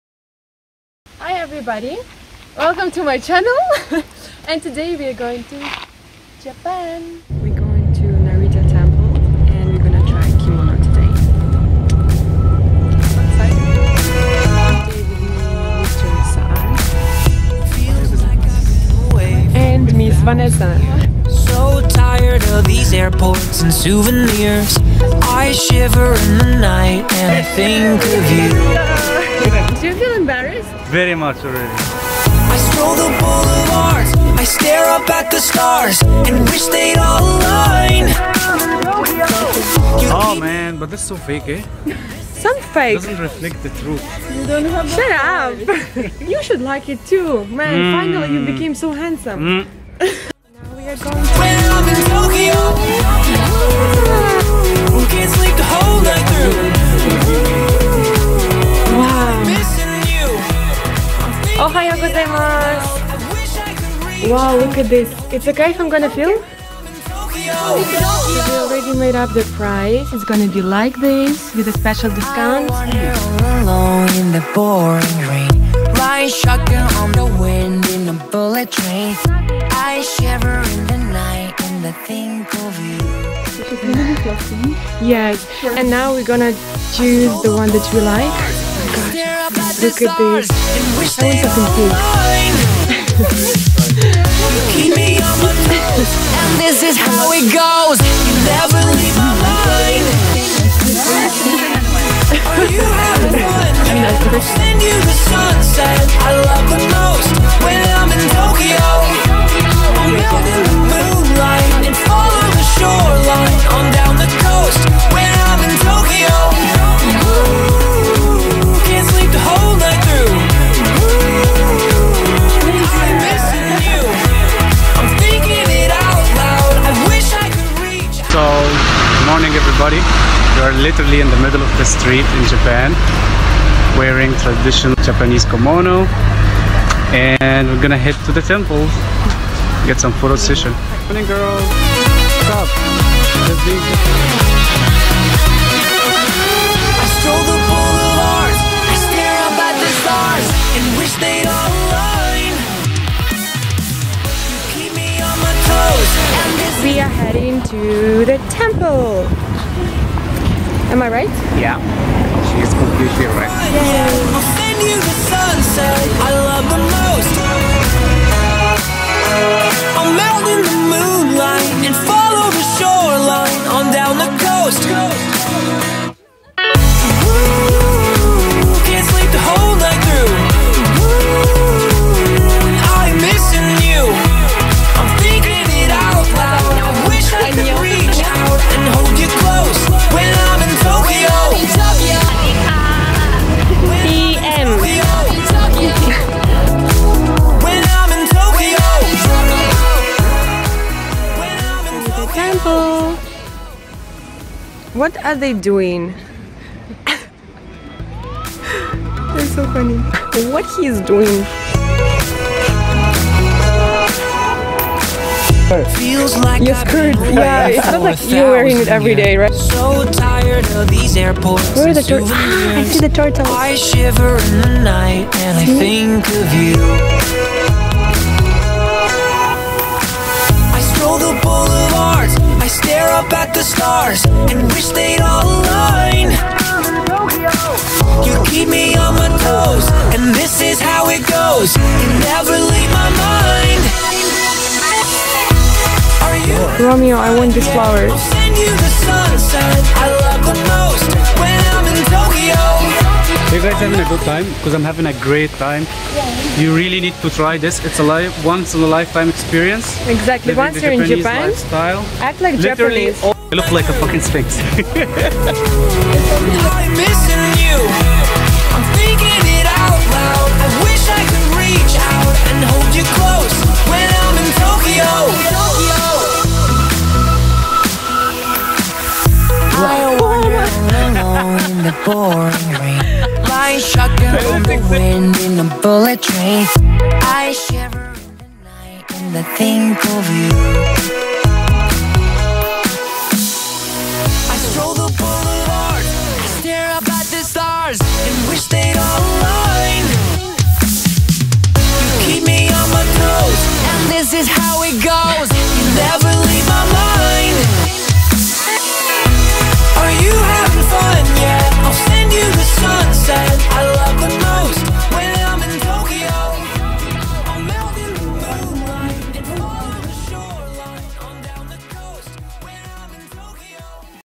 Japan, Narita-san Temple, kimono rental experience
Japan is one of my favorite countries! If I have a chance to go there, I never miss it. This time we tried japanese traditional costumes, kimono! So beautiful! If you want to know more, ask me =)